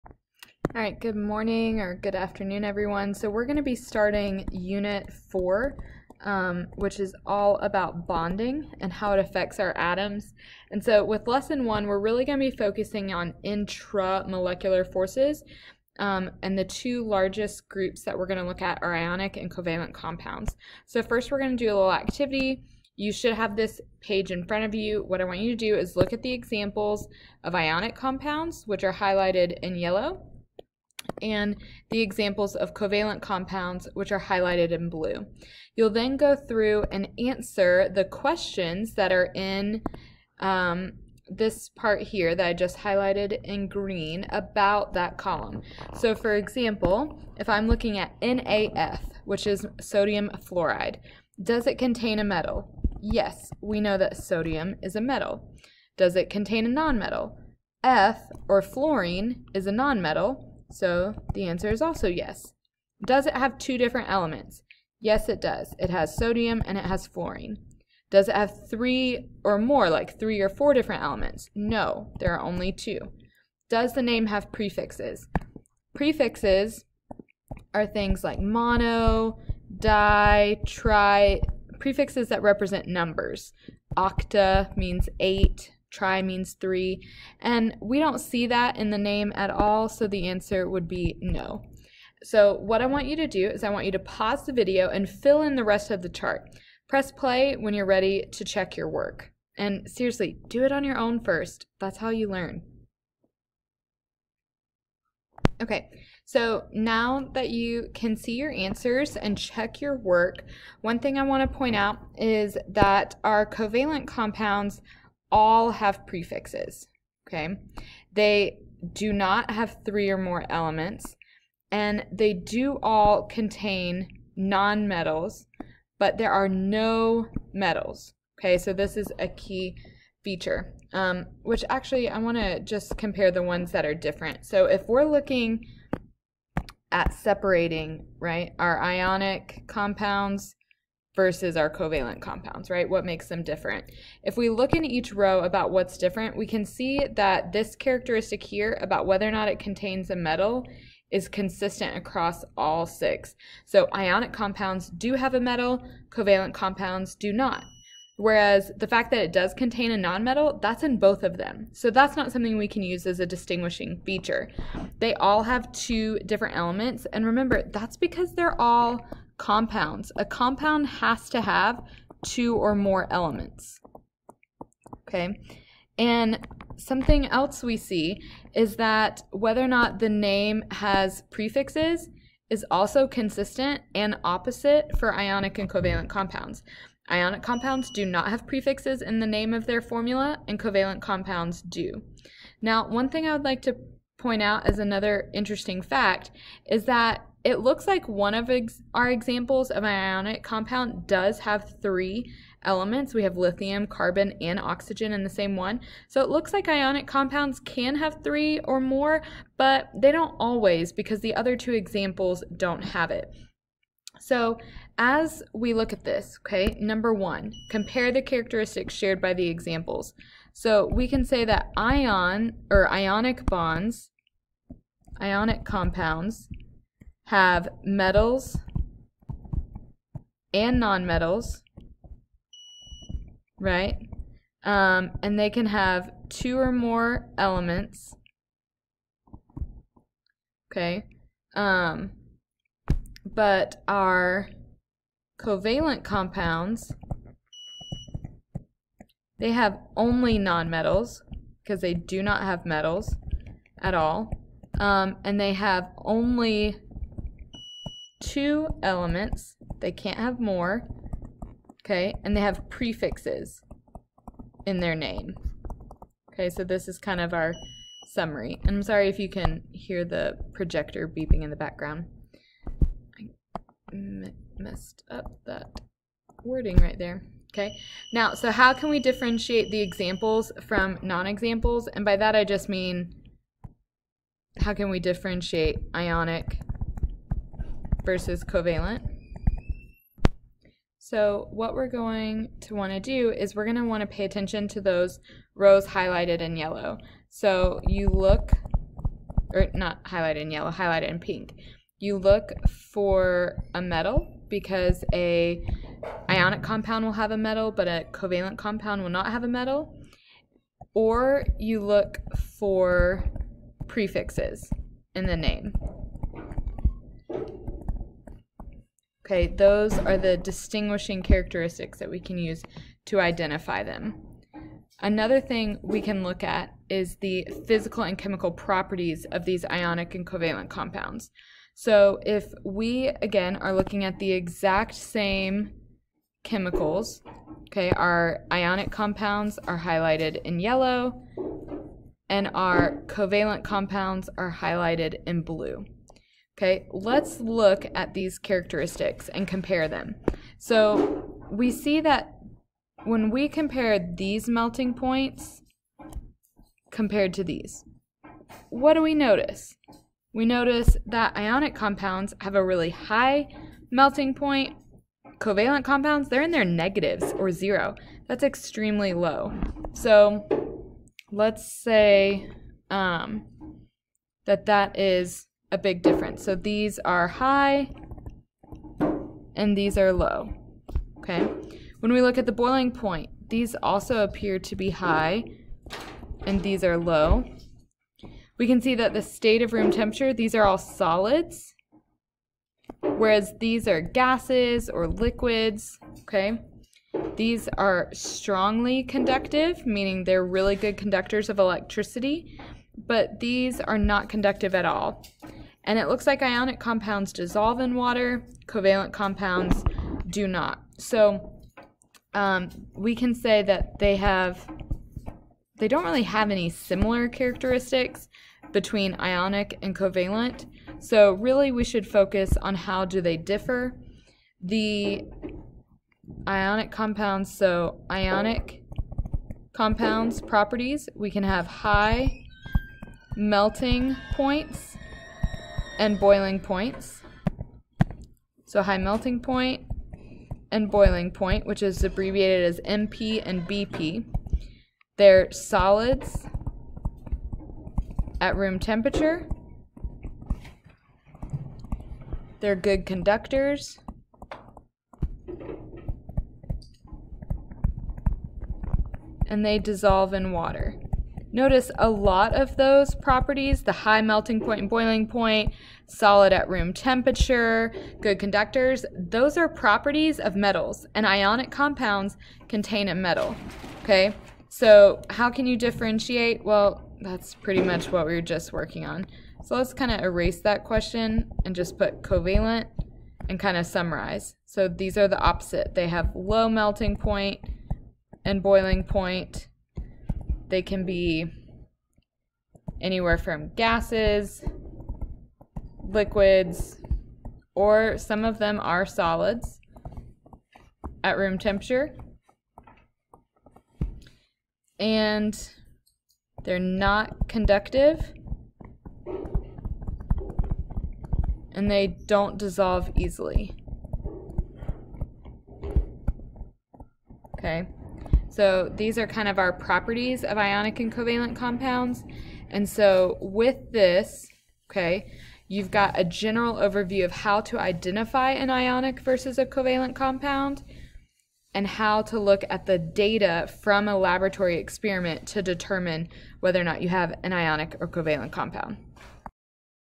21-22 Ionic vs Covalent Identification & Properties
This video reviews how to determine if a compound is ionic or covalent. It also reviews the physical properties of the 2 types.